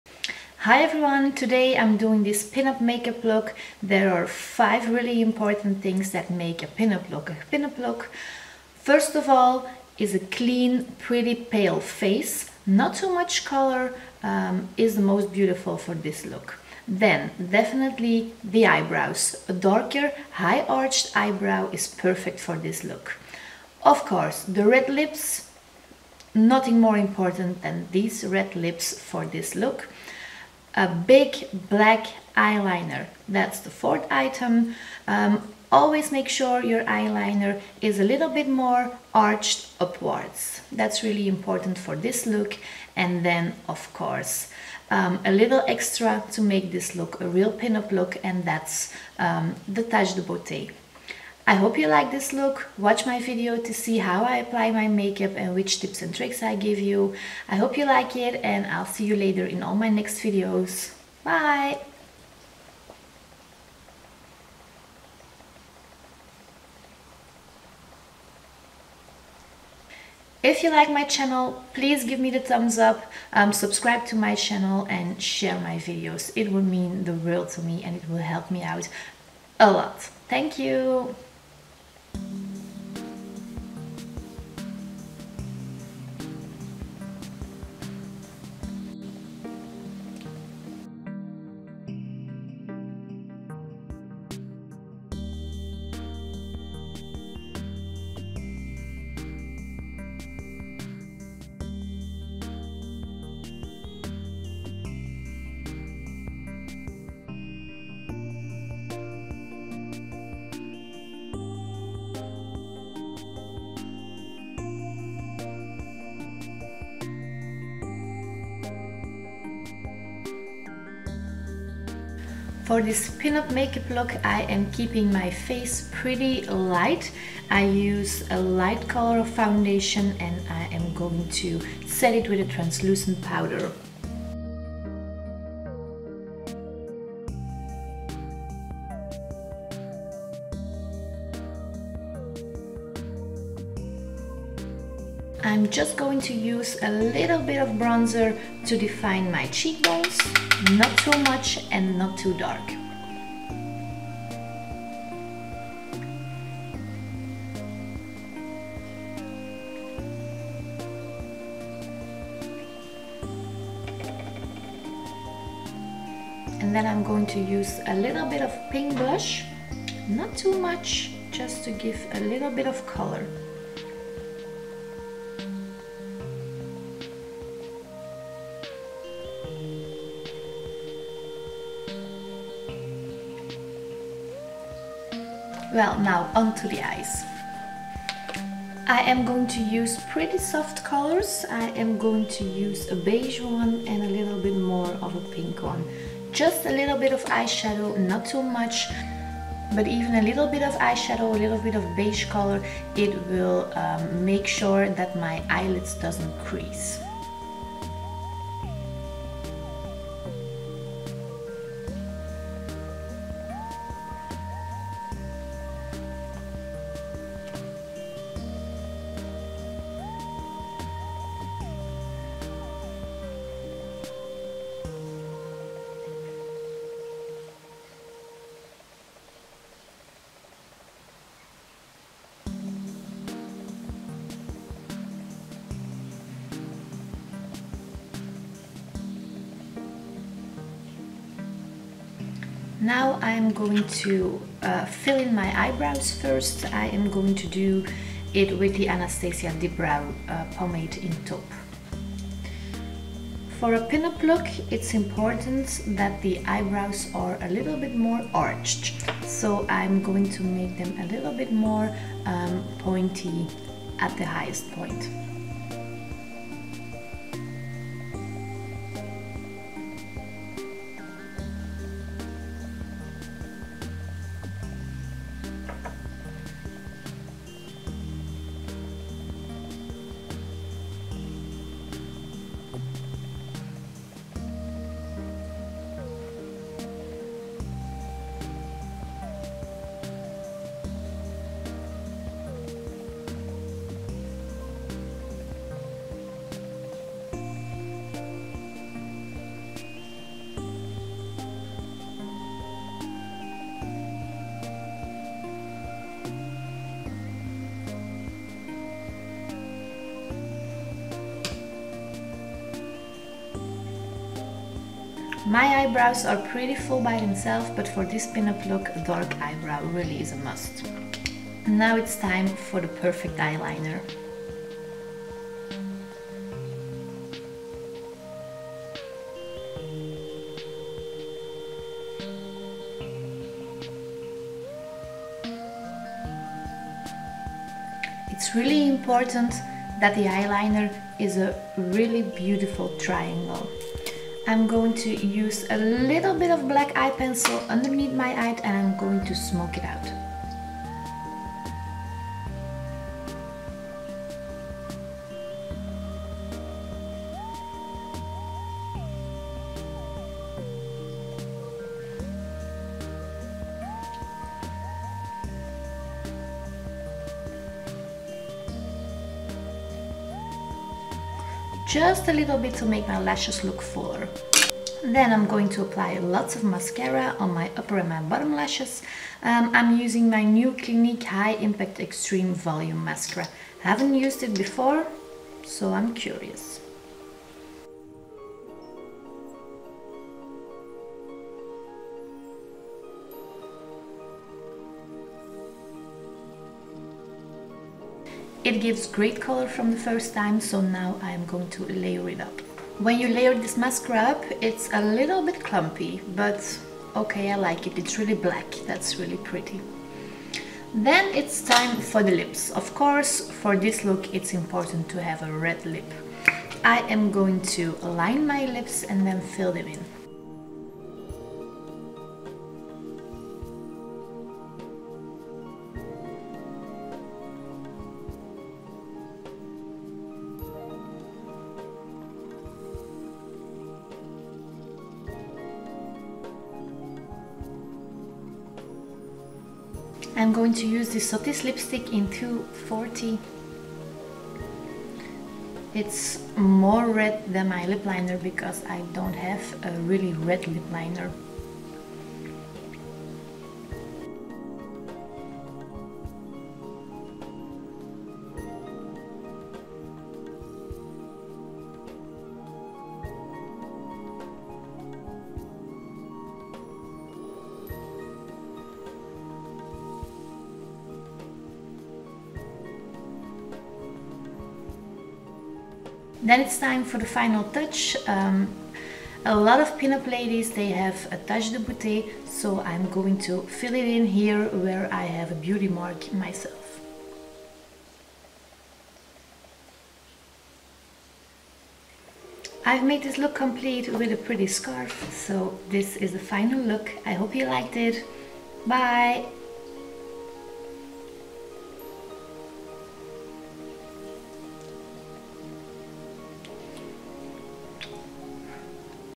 Pin Up makeup Tutorial
Hi Everyone,
I absolutely love how this makeup look turned out
There are 5 important things that make a pin up look a pin up look:
Check out this video to learn what these are.

Products I've used:
Estee lauder maximum cover
L'oréal le teint accord parfait N4
Sephora transluscent powder
Hoola bronzer
ArtDeco moonlight blush
Sephora makeup bag palette
ArtDeco long lasting liquid liner 1
Clinique quickliner for eyes intense
Clinique high impact extreme volume mascara First time I used this mascara! REVIEW
Bourjois lippencil 05 berry much
Sothys lipstick 240
ArtDeco eyeliner brown for the beauty mark

Have you seen my latest makeup tutorial?

xx Caroline